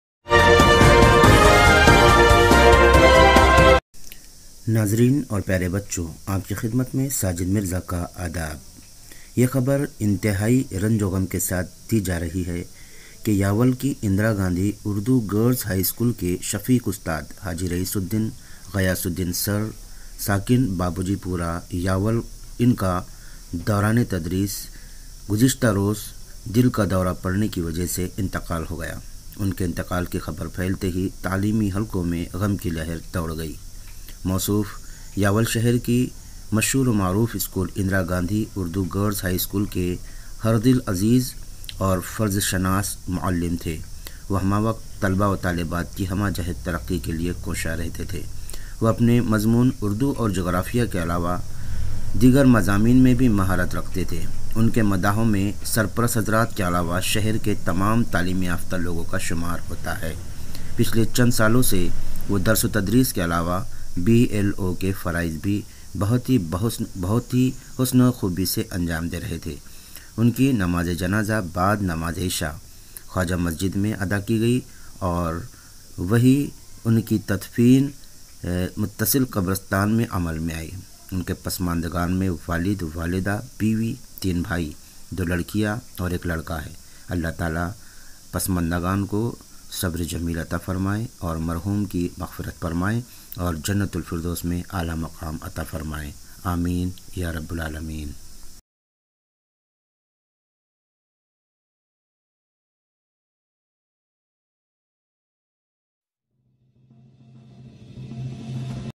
11Dec:یاول کے ہر دلعزیز مدرس حاجی رئیس الدین سرکا انتقال
Khandesh Urdu news